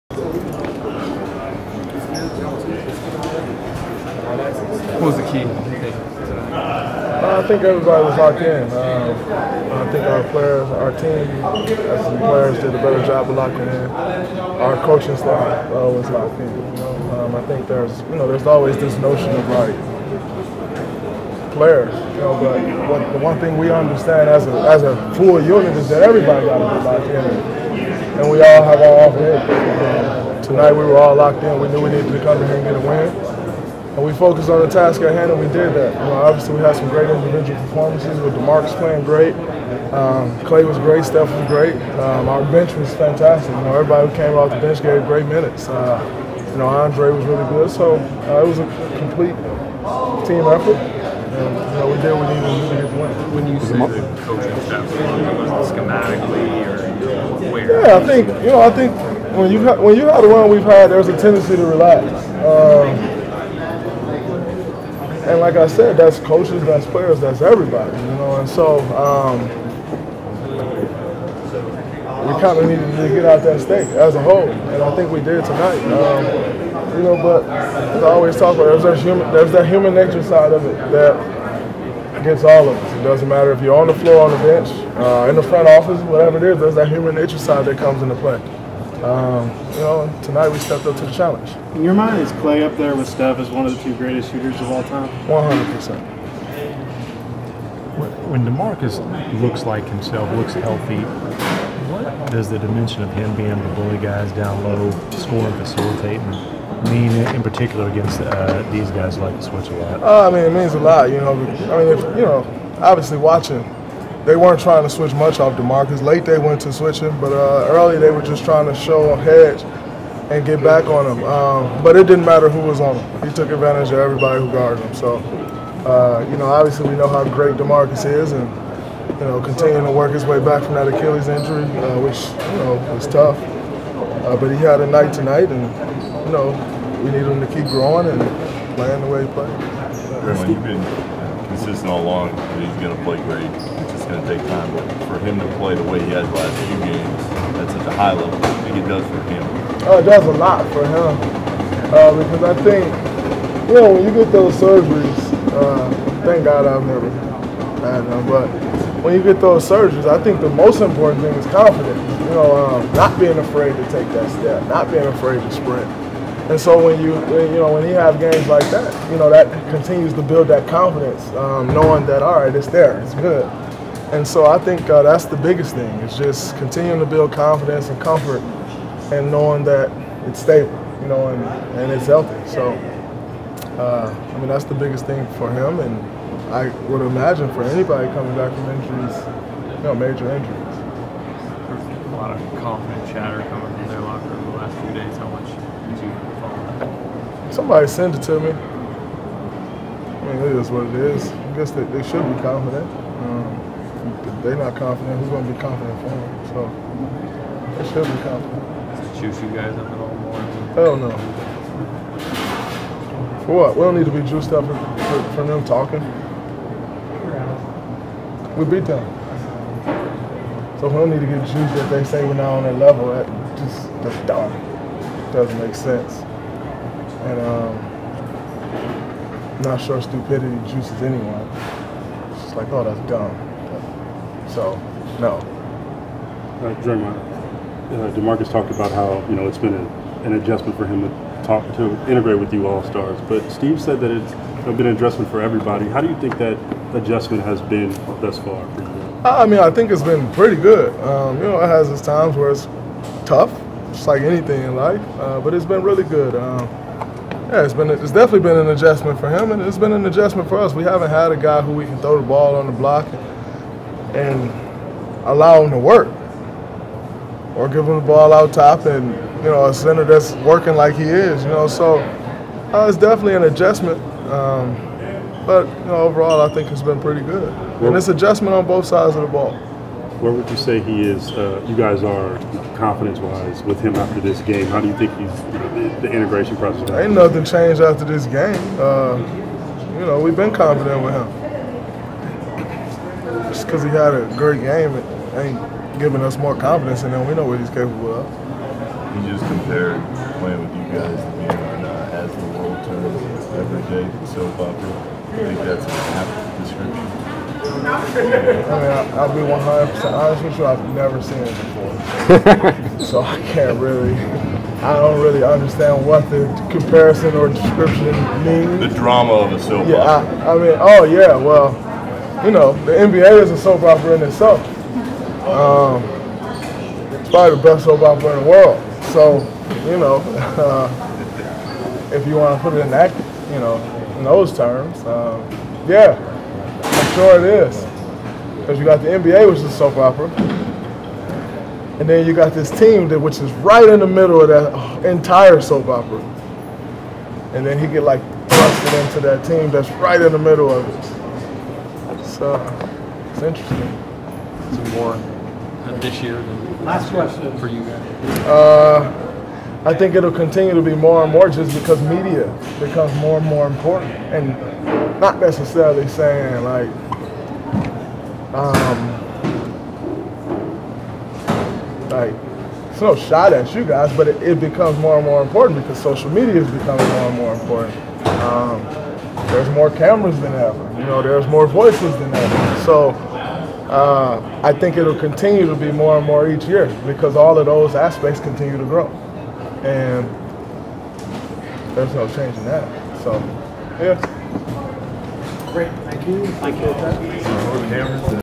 Entire DRAYMOND postgame: NBA "best soap opera" in world; Boogie Cousins confidence after surgery
From Golden State Warriors (46-21) postgame after Rockets, at Toyota Center in Houston, TX.